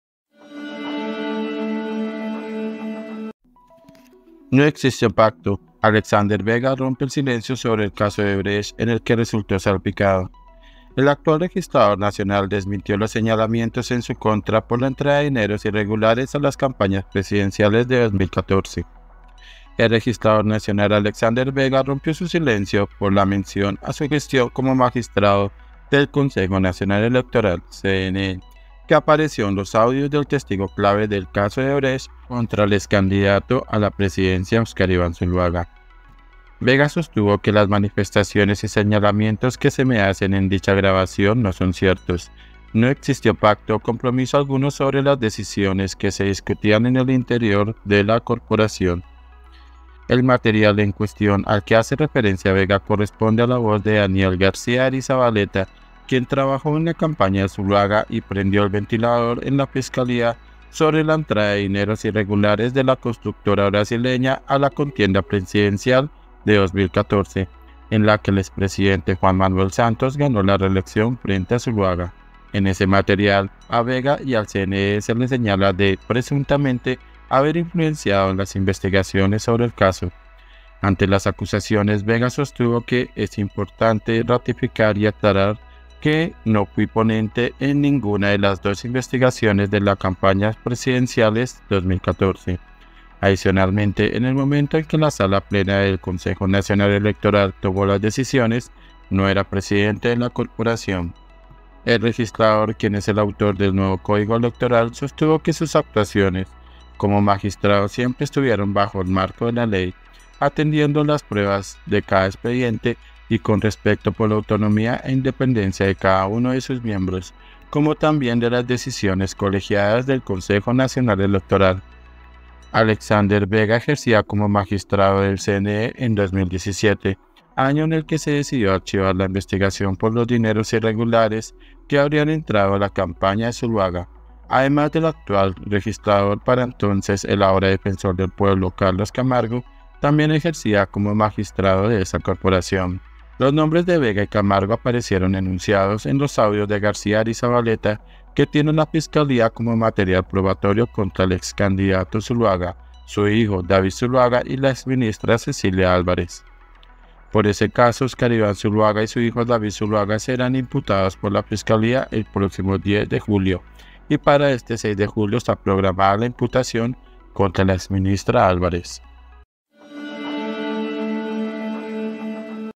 🔴 ALEXANDER VEGA ROMPE EL SILENCIO SOBRE CASO ODEBRECHT
El actual registrador nacional desmintió los señalamientos en su contra por la entrada de dineros irregulares a las campañas presidenciales de 2014.